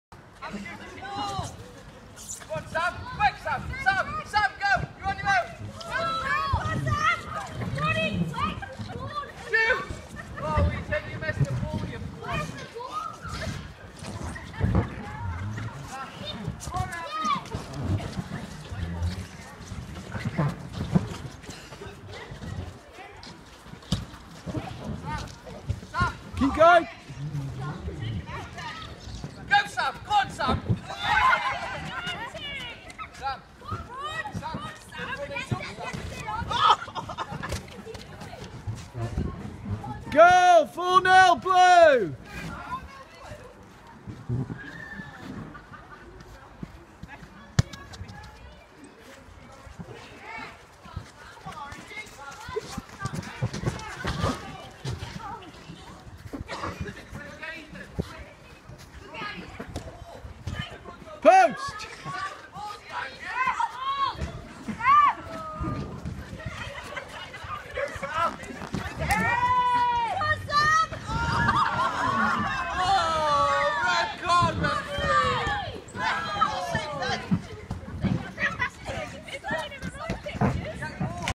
069
Children's Bubble Football Party in the Snow.  The Blues Slip and Slide Their Way To Victory as The Snow Falls in Stoke o Trent